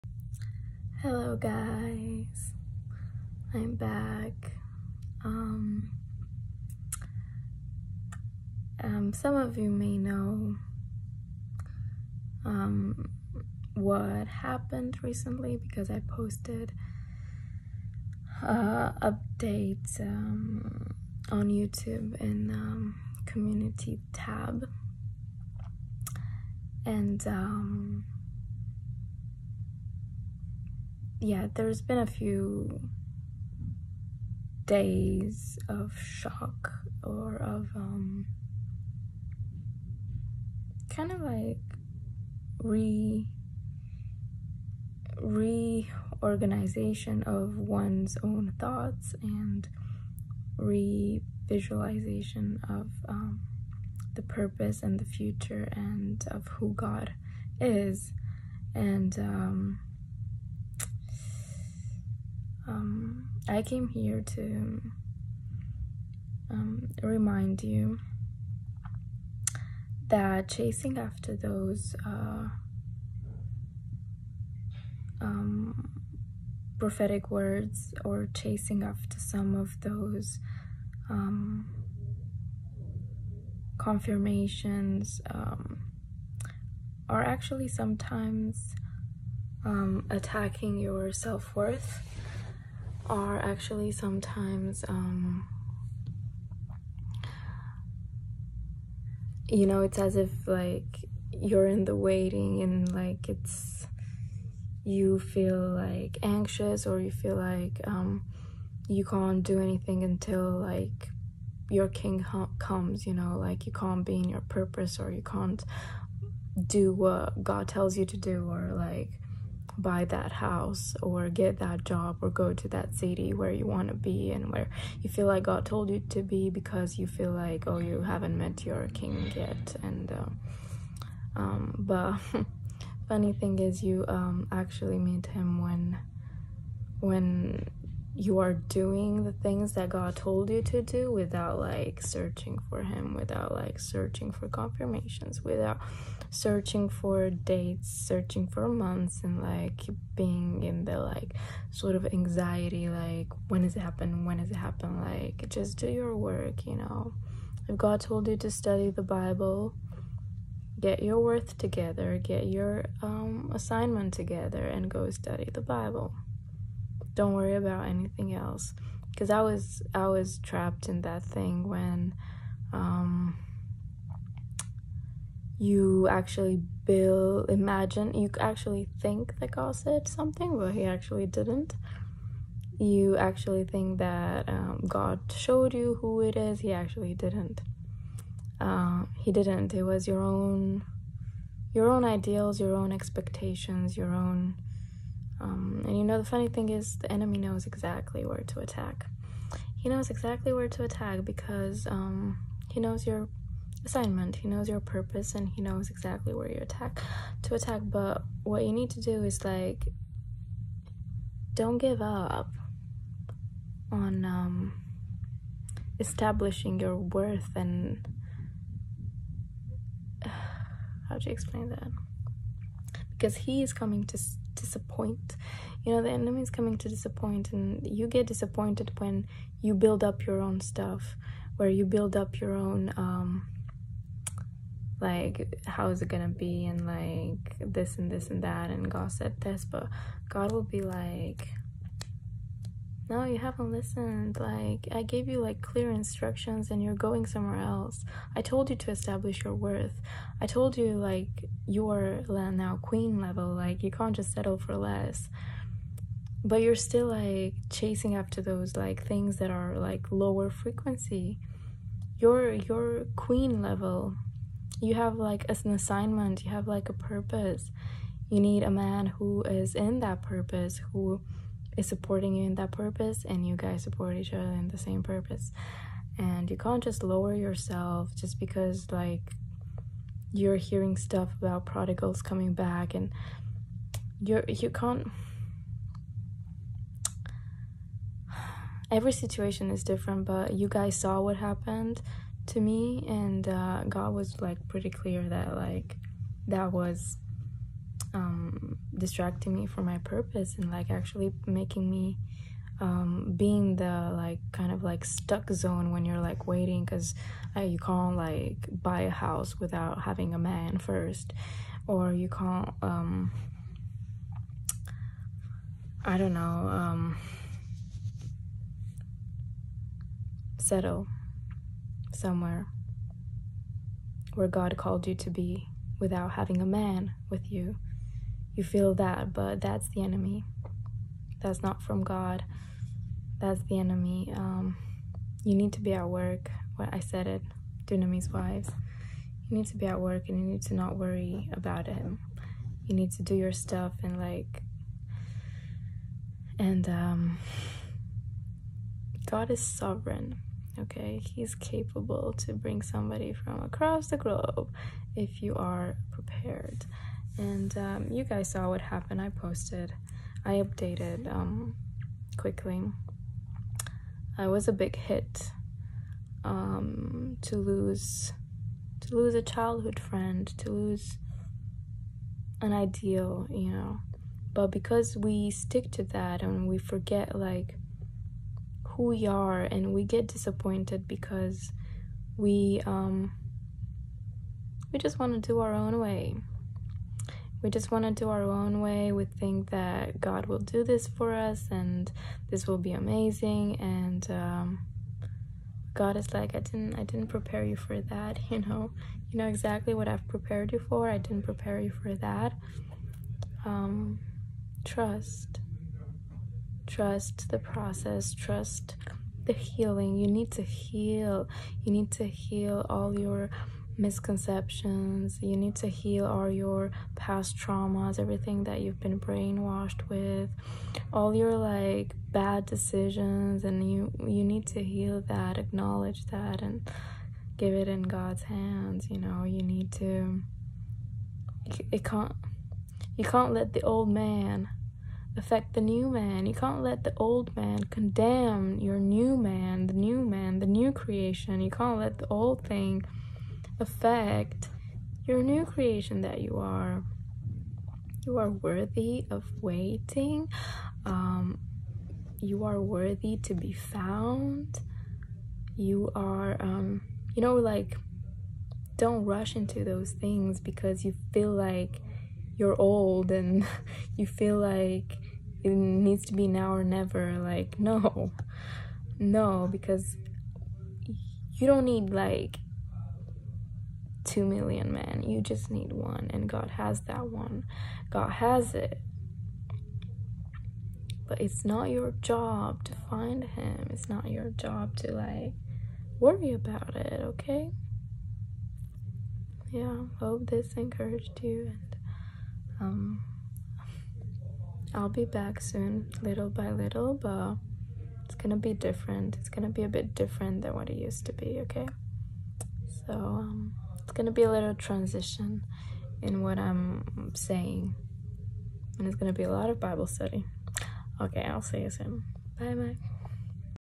“no baby, you didn’t get the point” - God | Stop chasing for confirmations and be
The one I thought was my kingdom husband, the one I thought God revealed to me during my worthy preparation season, the one I had dreams to marry... decided to take his life a few days ago... I’m so confused on whether I heard right... I don’t know what to do now how to RE-start. Everything I heard, or I thought I heard, about him coming on a donkey directed me in the wrong direction. Directed me towards a man, not towards God. And disappointment came. And the enemy knows your assignment and knows exactly where to attack. I thought I was in my promise land finally, my YouTube ministry exploded and I got a permanent job in a place I thought God directed me to (temporarily). Now there’s a death on my hands because I was the only family he had in this country... I was supposed to be the light, but the enemy’s attacks were too strong on him and he gave up... remember he comes to steal, kill and destroy. Not only lives but your purpose too. Don’t let that happen. Don’t let it take you off guard and confuse you.